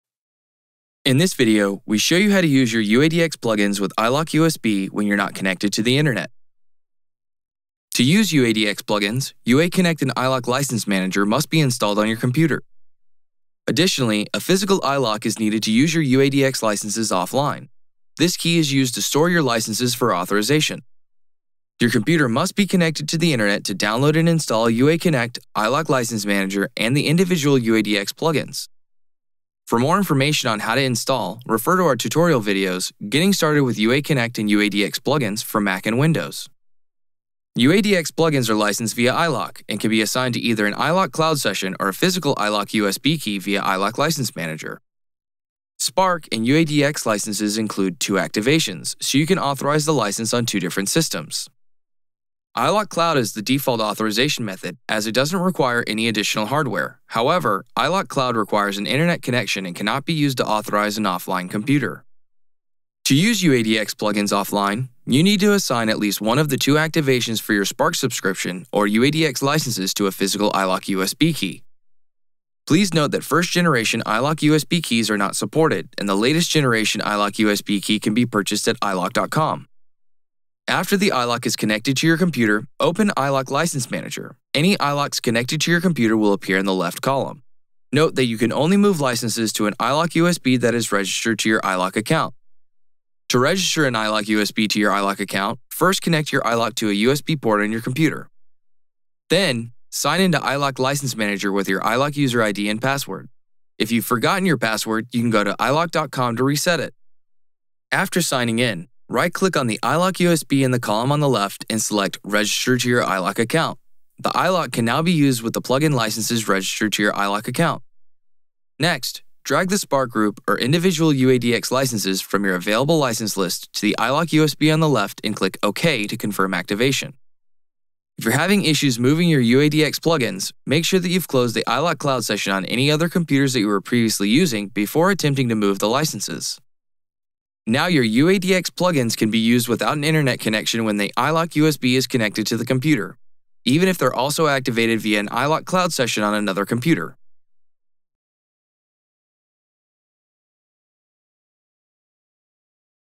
UA Support: How to Use UAD Native Plug-Ins Offline
Watch this short video to learn how to use UAD Native plug-ins offline via iLok.

Learn more by visting our knowledge base at help.uaudio.com